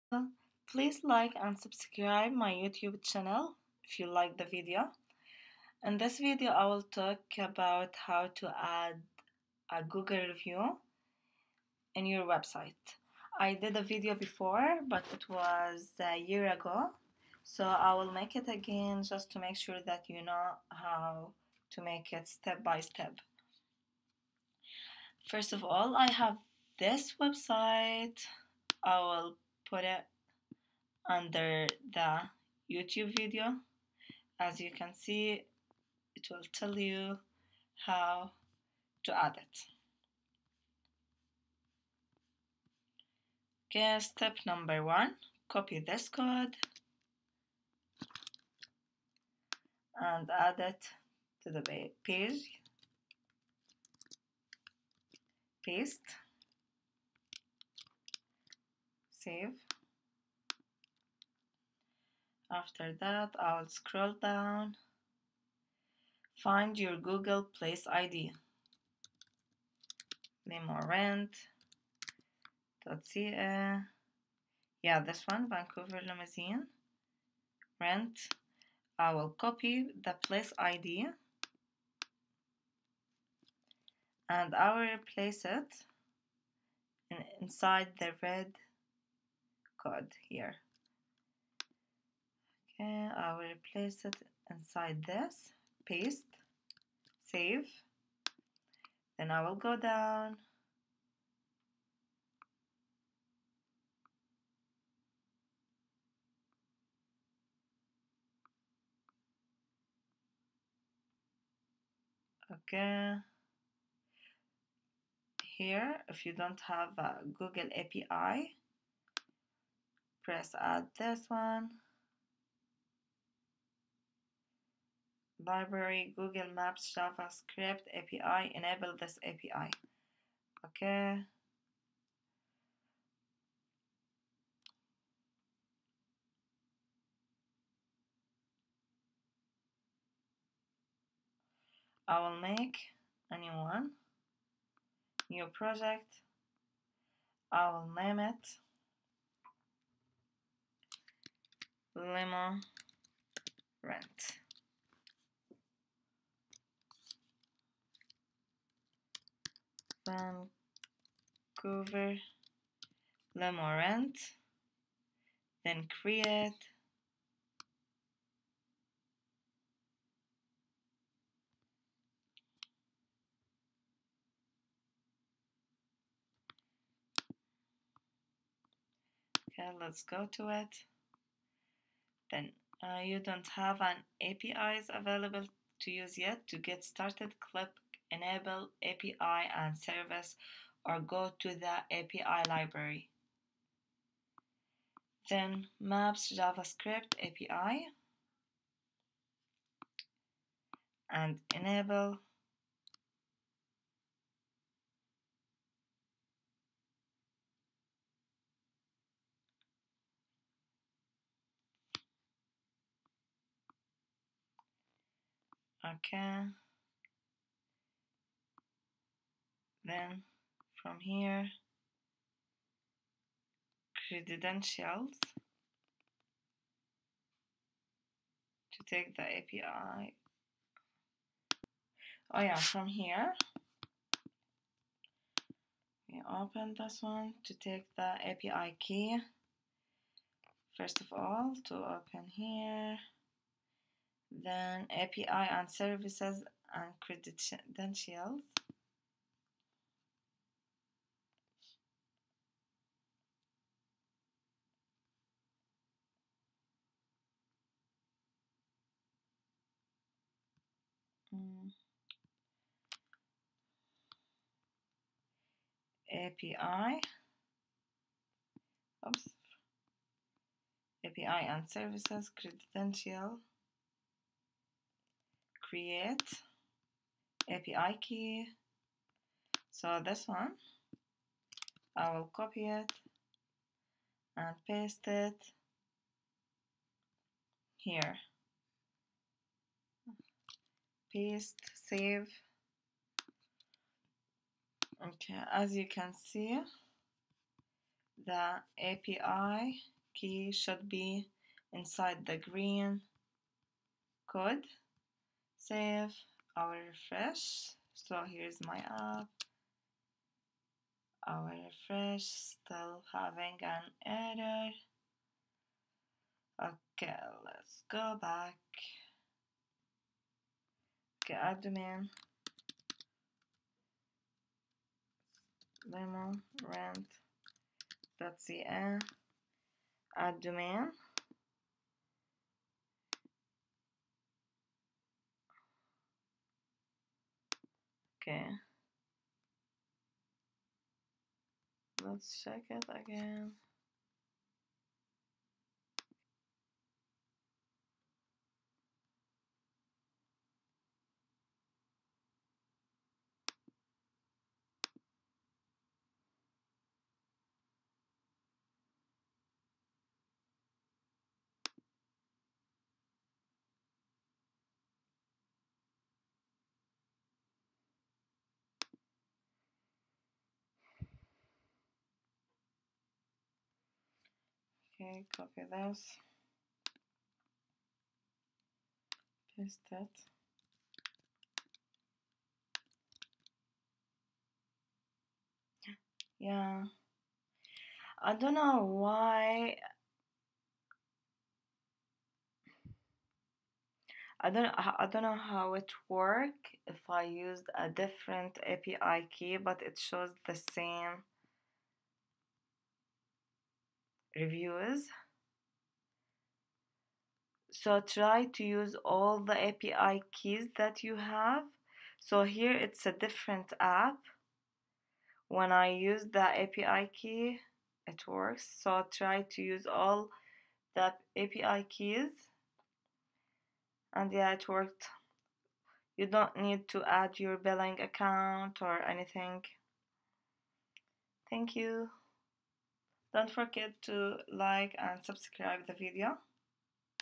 Add google review to your website 2020
Quick video of how to add google review to your website with finding the placeId and the API key to your website